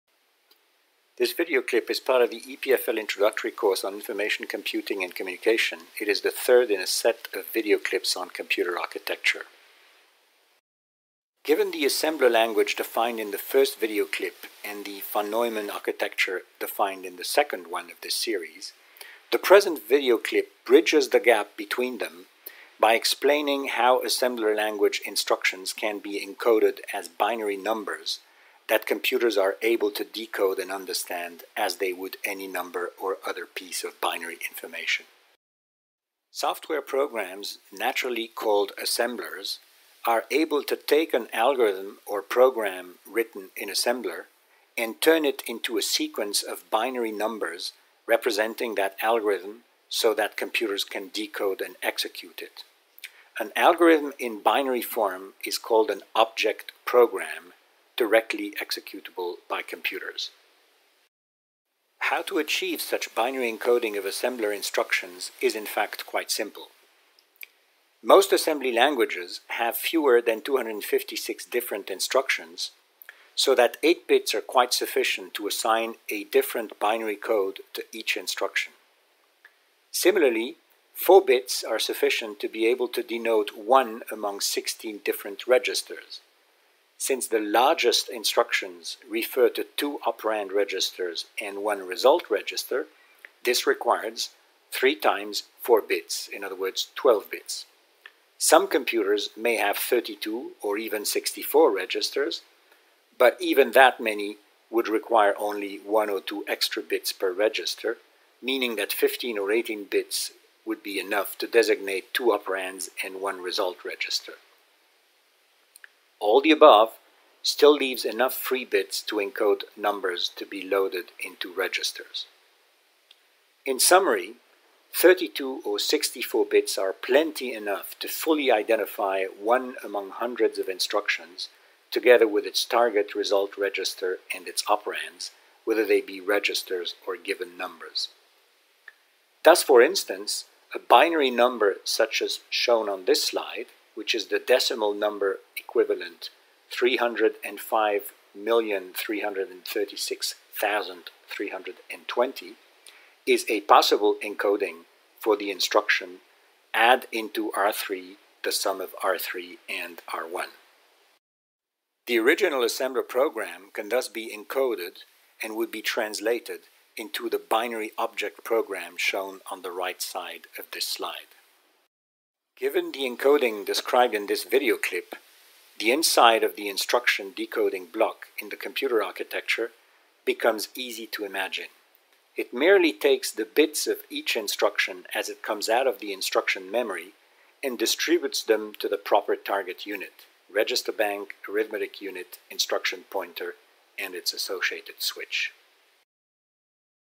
How are Instructions Encoded inside a Computer? Philippe Janson
This clip is part of the elementary course on Information, Computing & Communication (ICC) of the Ecole Polytechnique Fédérale de Lausanne, Switzerland.  It explains how computing instructions are encoded inside a computer.